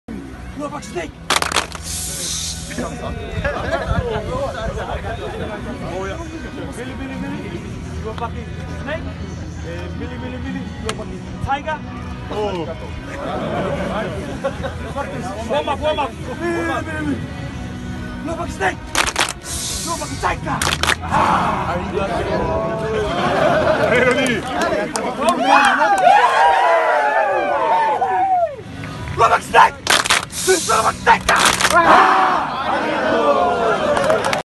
La superbe célébration d'après-match des clermontois avec les joueurs de Top League
Très belle image des joueurs de l’ASM Clermont qui célèbrent la fin de ce match en communion avec leurs adversaires de la Top League 👏